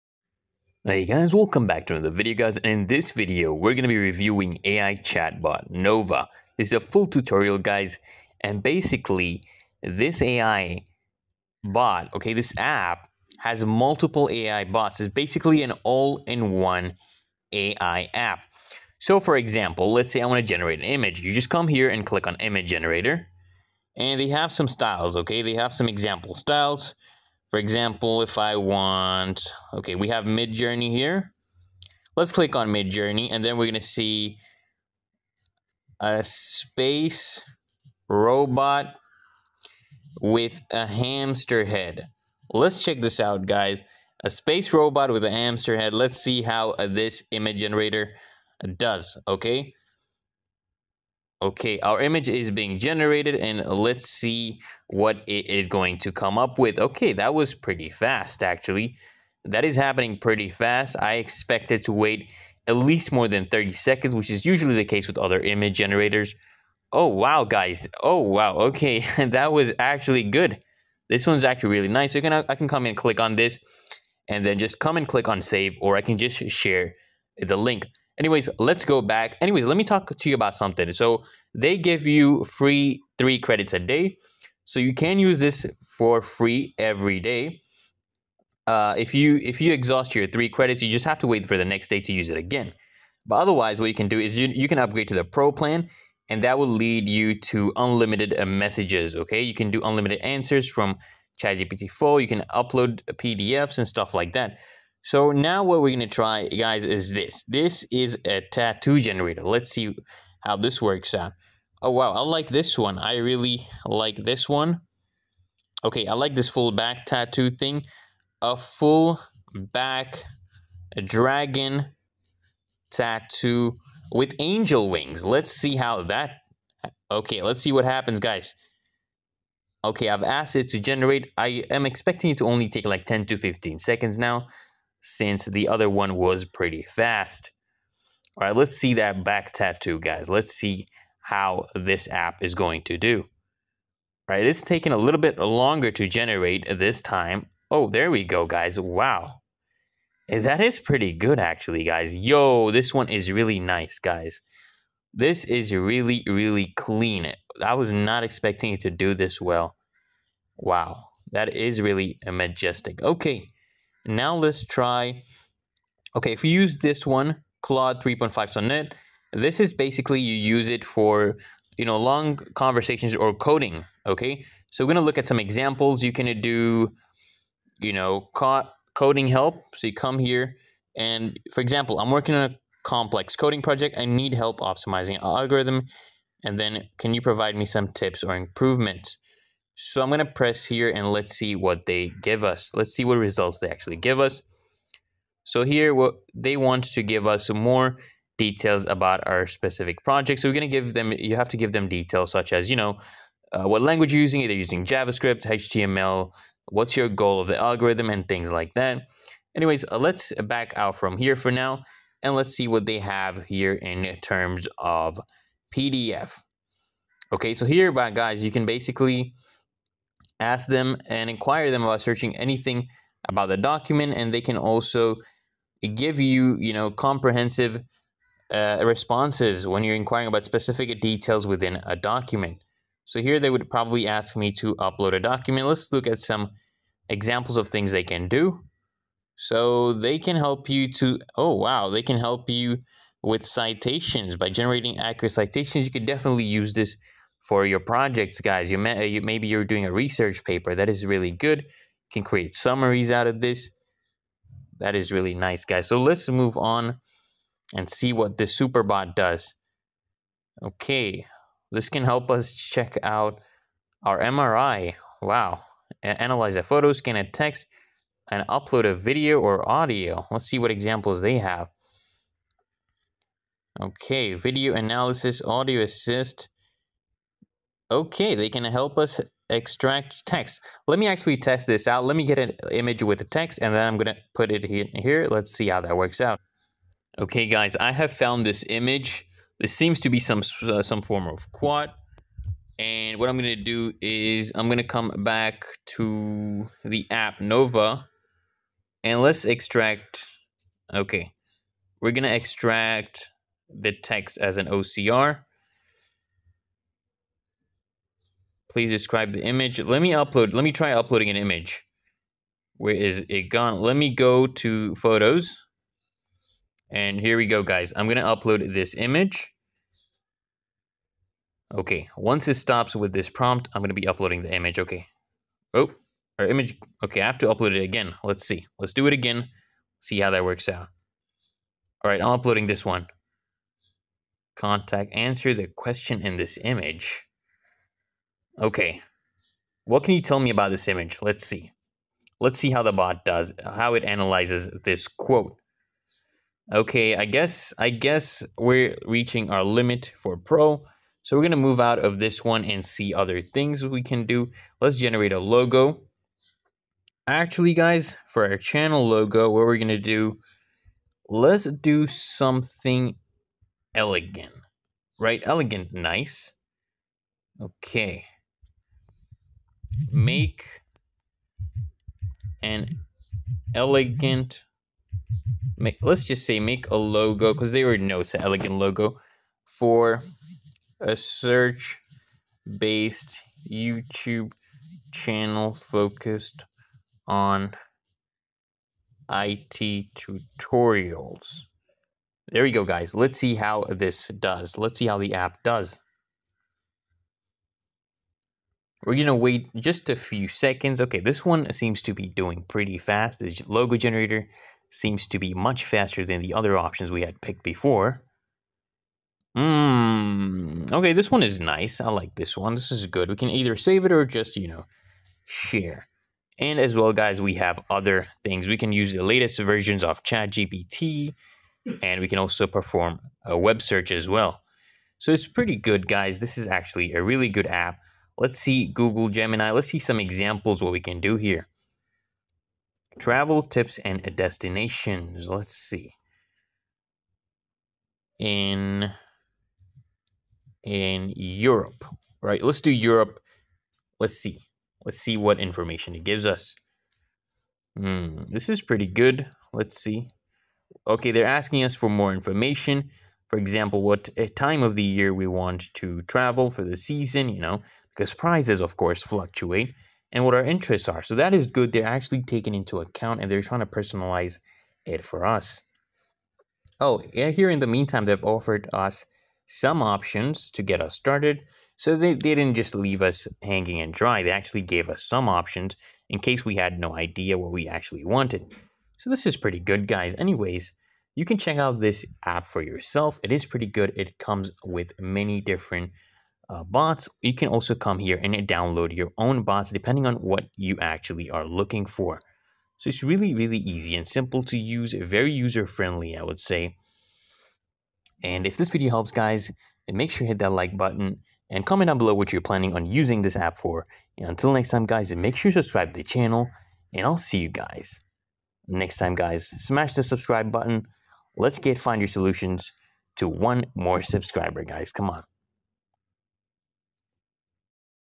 OVER POWERED! AI CHATBOT NOVA (Full Tutorial) - 2025
If you're looking for a video on how to use Nova AI chatbot, here you go! In this video, I'll show you
social nova chatbot vs manychat. Make sure you watch the video till the end because you'll learn nova ai chatbot commercial. It's easy and simple to do from your phone or laptop.
AI CHATBOT NOVA (Full Tutorial) - 2025